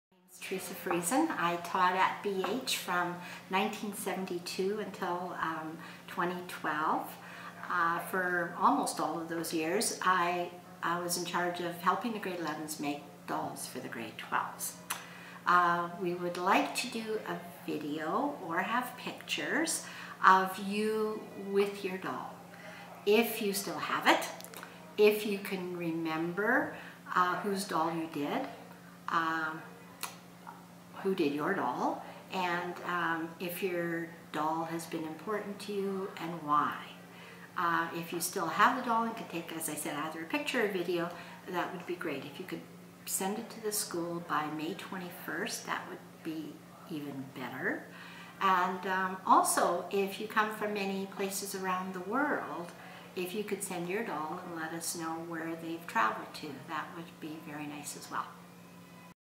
Doll Assemby Video Requests
Retired BH Teacher Mrs. Friesen asks alumnae to send in video messages featuring themselves, their dolls, and their special memories.